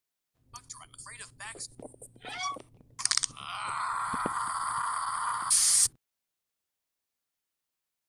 LPS: I'm Afraid Of Backs!
Abe: Doctor, I'm afraid of backs!-

Dr. Richard: 😈 *snapped and twisted his neck*

Abe:AAAAHHHHHHHHHHHHHHHAAHAHAHAH!